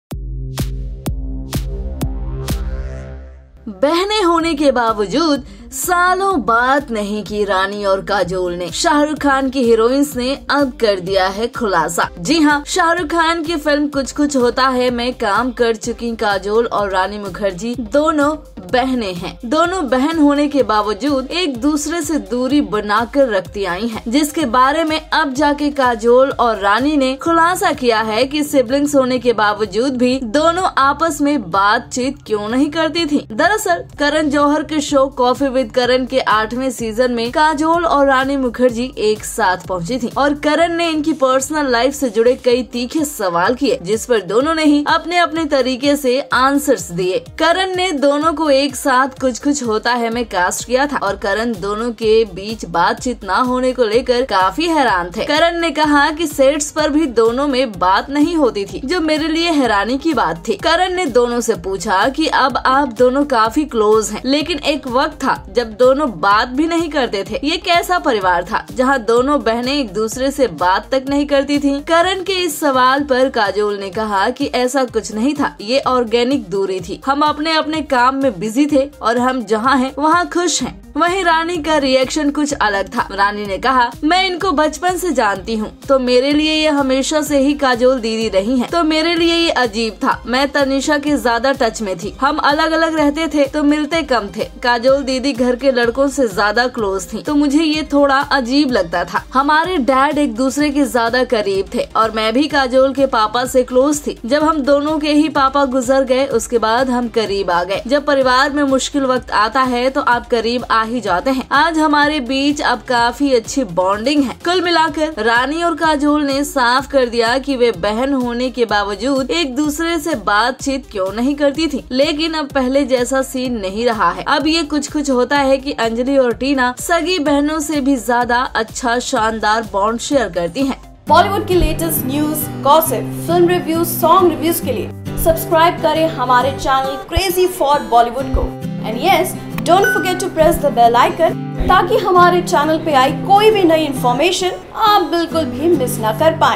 Despite being sisters, Rani and Kajol did not talk for years.
Kajol And Rani Mukerji Open Up About Why They Weren’t Speaking To Each Other